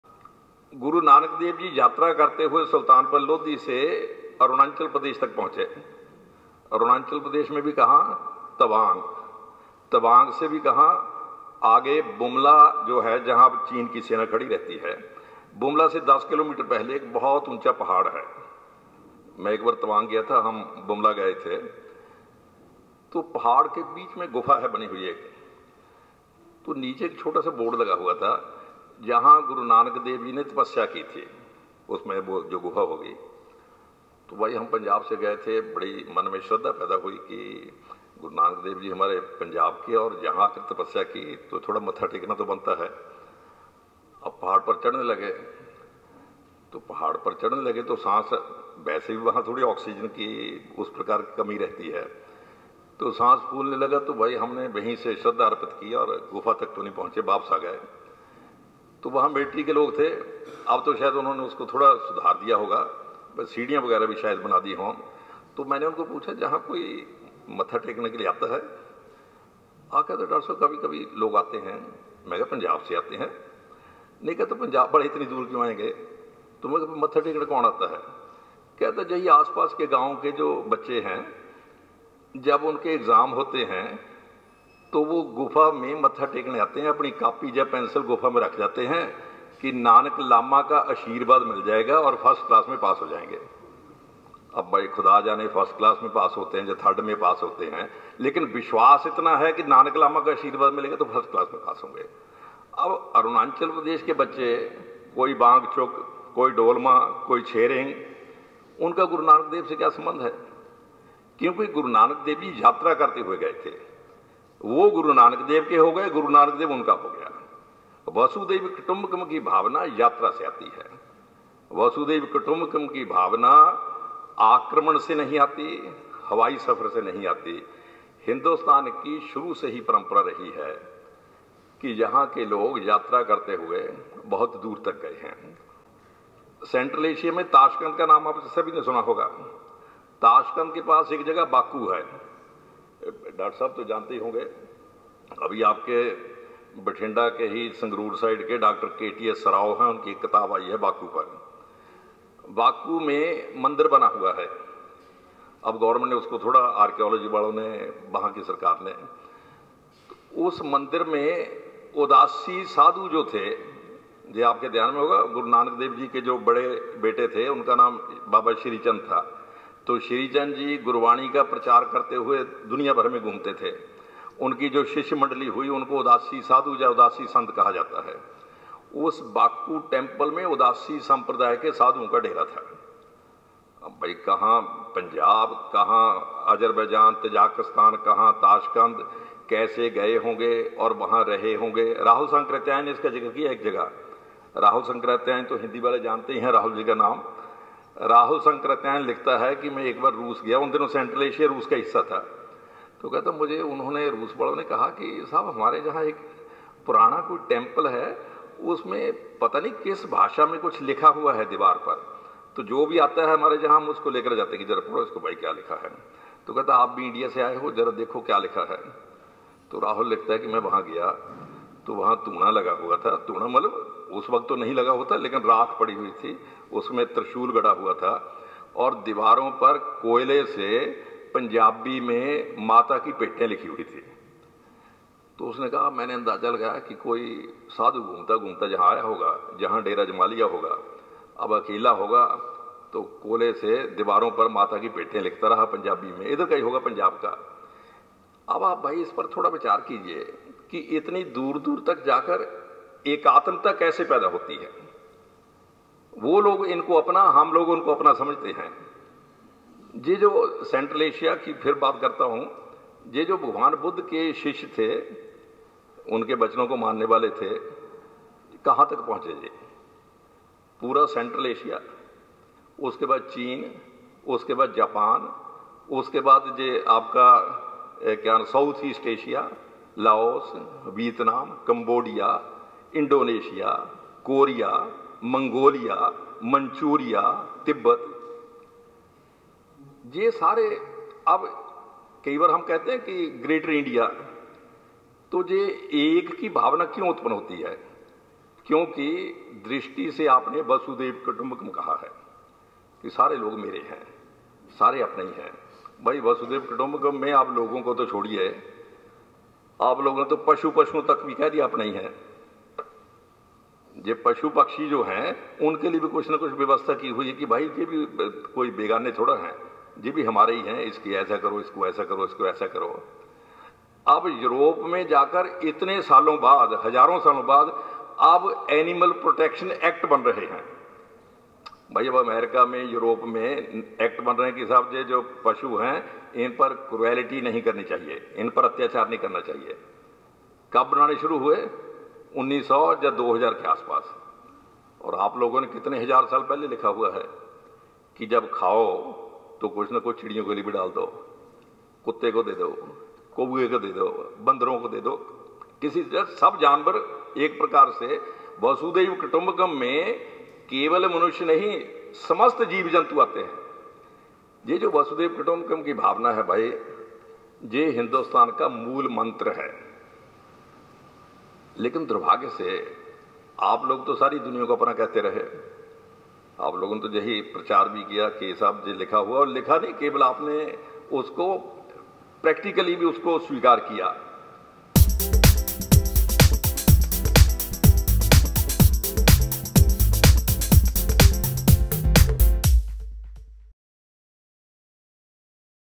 ਸੁਣੋ Shri Guru Nanak Dev ji ਦੀ ਮਹਿਮਾ ਅਤੇ ਸਮਝੋ G20 logo ਦਾ meaning
Pro. Kuldeep Chand Agnihotri explained the meaning of the G20 logo tagline and how Guru Nanak Dev Ji is the guru of people in different parts of the earth.